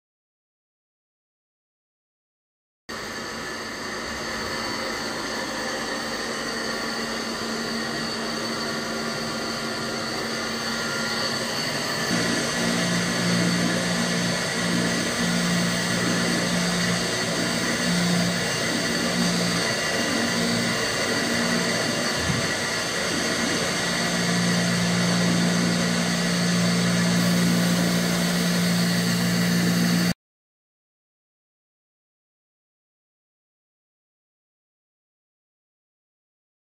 Milling a radial hole on the EMCO 325II with Siemens Shopturn
had to do a test for milling a radial hole on the EMCO 325II with Siemens Shopturn...
the hole is far from perfect but it work..